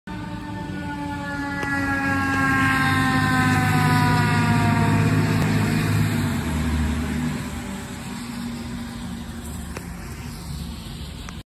Montville NJ Engine 352 Responding 8-22-21
Responding Mutual aid to ￼ Towaco for a Co Alarm